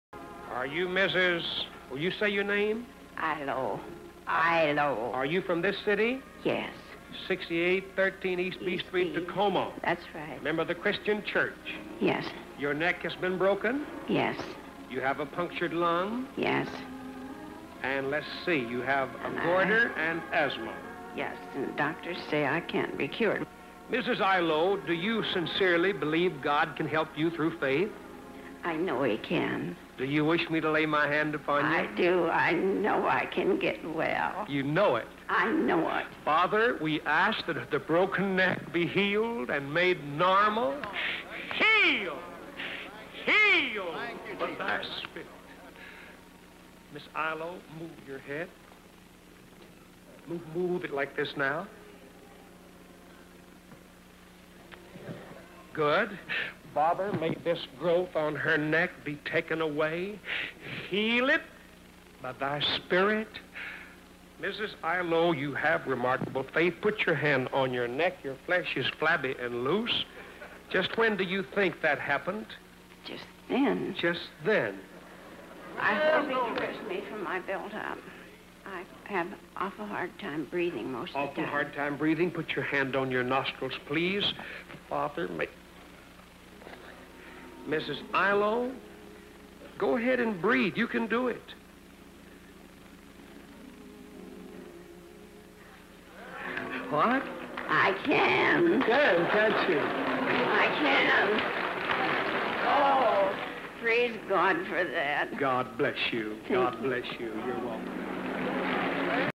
Молитва-исцеления шеи Орал Робертс
Крусейд Орала Робертса, Такома, Вашингтон 1956 год.
Эзра, была сломана шея.